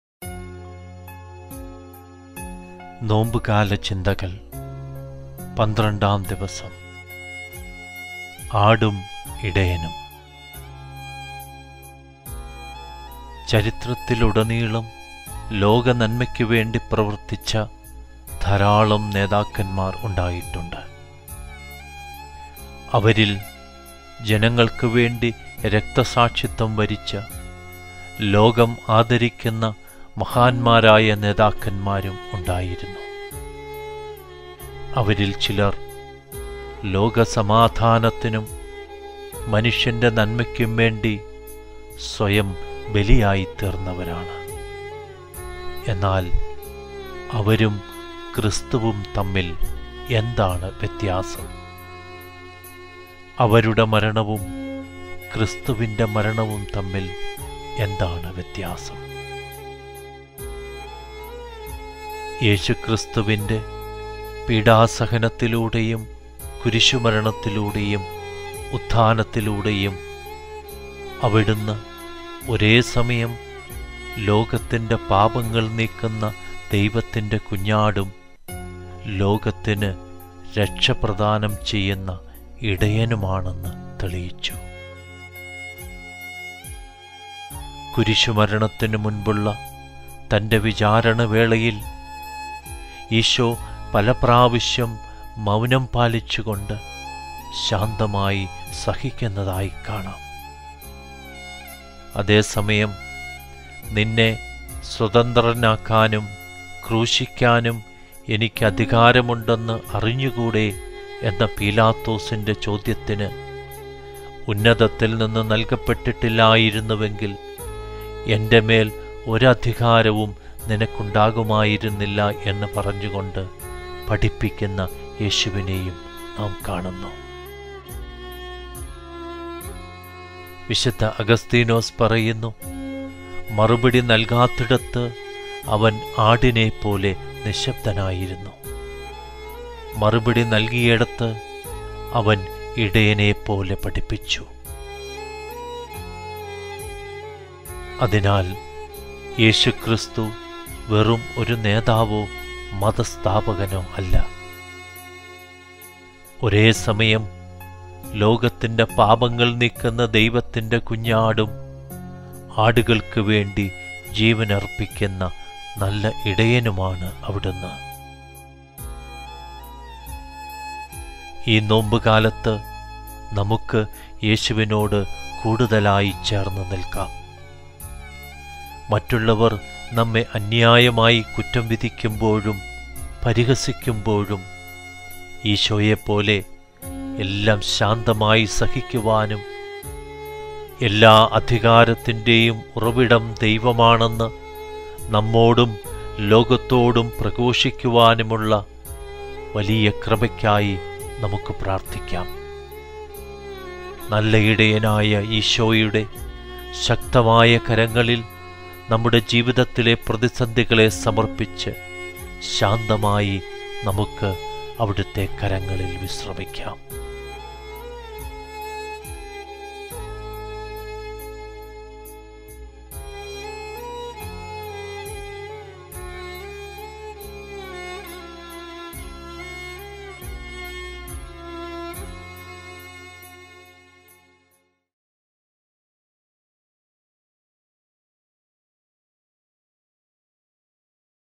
"ആടും ഇടയനും” | നോമ്പുകാല ചിന്തകൾ | പന്ത്രണ്ടാം ദിവസം | Pravachaka Sabdam
ജനങ്ങൾക്കുവേണ്ടി രക്തസാക്ഷിത്വം വരിച്ച, ലോകം ആദരിക്കുന്ന മഹാന്മാരായ പല നേതാക്കന്മാരും ചരിത്രത്തിൽ ഉണ്ടായിരുന്നു. അവരിൽ ചിലർ ലോകസമാധാനത്തിനും മനുഷ്യന്റെ നന്മക്കും വേണ്ടി സ്വയം ബലിയായിത്തീർന്നവരാണ് എന്നാൽ അവരും ക്രിസ്തുവും തമ്മിൽ എന്താണ് വ്യത്യാസം? അവരുടെ മരണവും ക്രിസ്‌തുവിന്റെ മരണവും തമ്മിൽ എന്താണ് വ്യത്യാസം?
'പ്രവാചകശബ്ദം' ഒരുക്കുന്ന നോമ്പുകാല ചിന്തകളുടെ പന്ത്രണ്ടാം ദിനം കേൾക്കാം. ധ്യാനിക്കാം.

🔹 *കൂടുതൽ  വീഡിയോകളും ക്രൈസ്തവ വാർത്തകളും ലേഖനങ്ങളും ഉടനടി ലഭിക്കുവാൻ പ്രവാചകശബ്ദത്തിന്റെ പുതിയ ഫേസ്ബുക്ക് പേജ്_  - Pravachaka Sabdam News ദയവായി ലൈക്ക് ചെയ്യുക. ഫോളോ ചെയ്യുക*